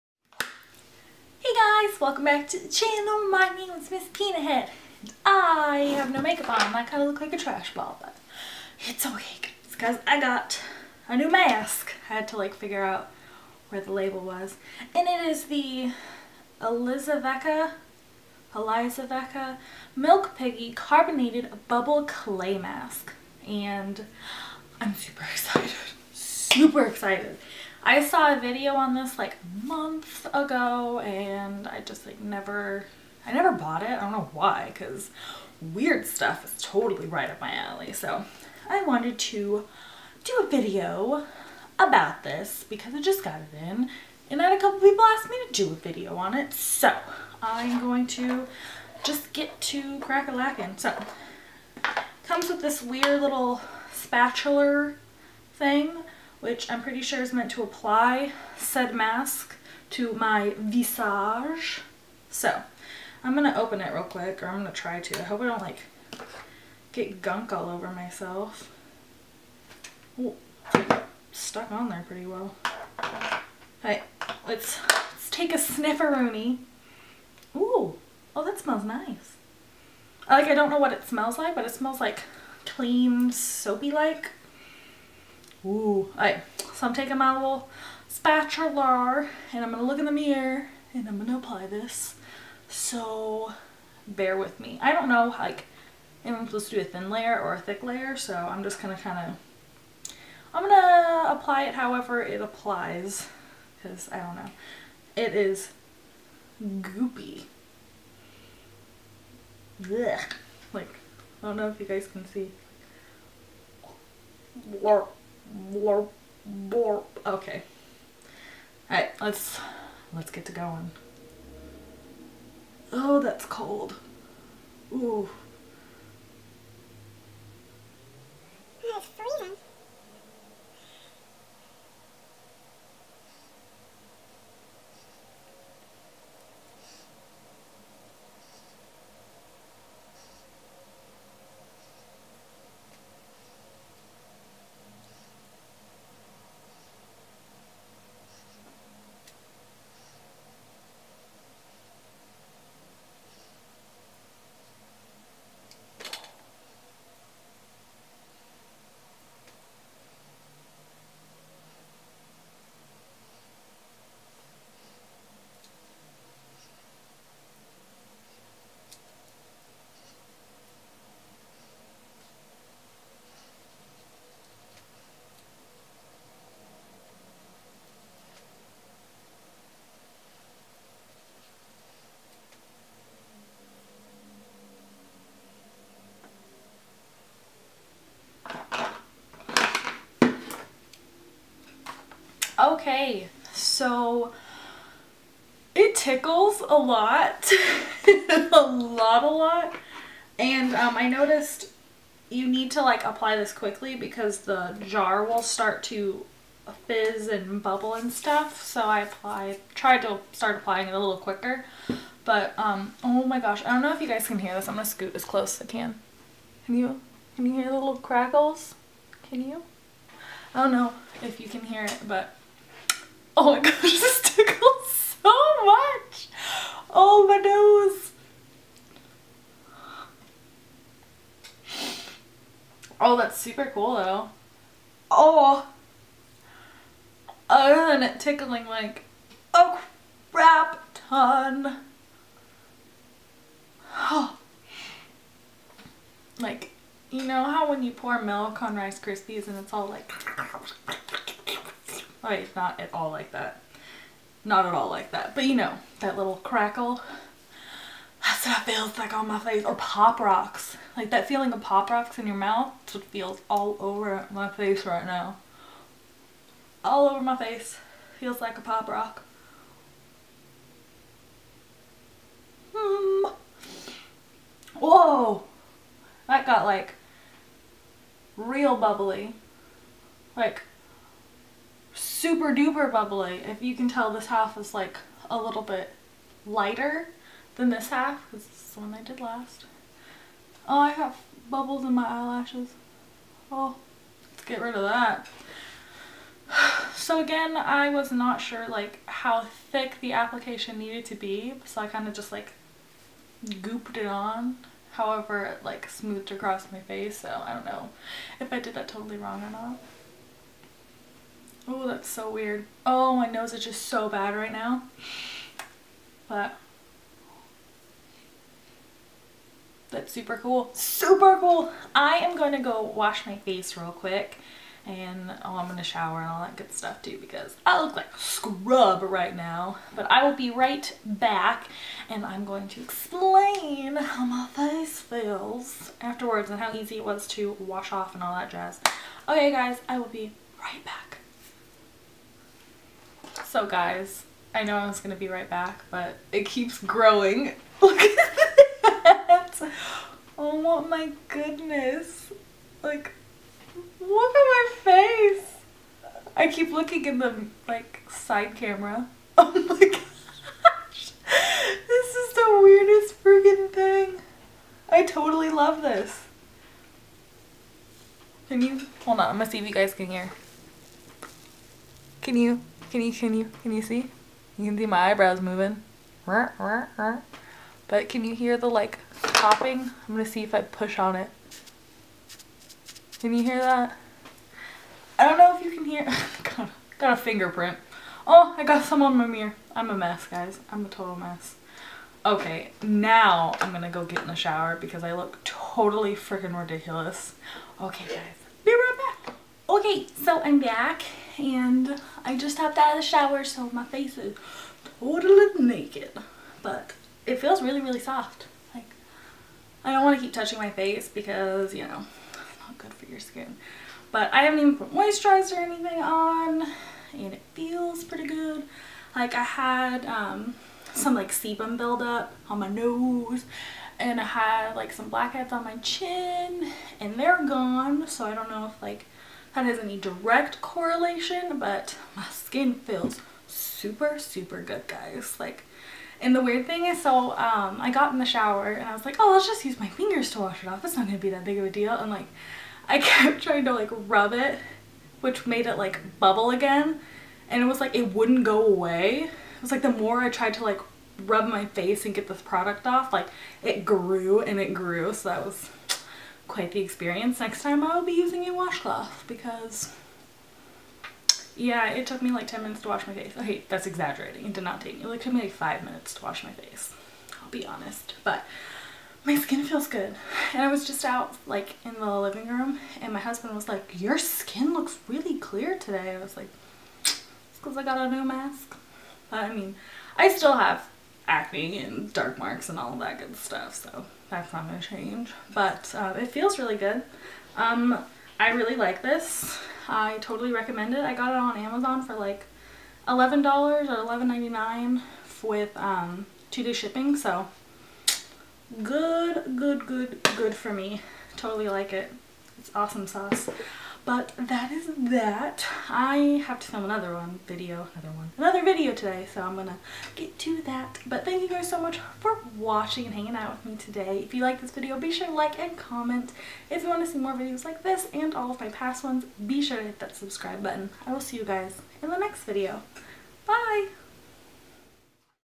Bubble Clay Mask Review!
This mask is SUPER weird and I totally love it!! I love finding weird things and this is very weird. I got it on amazon and it was fairly cheap so score!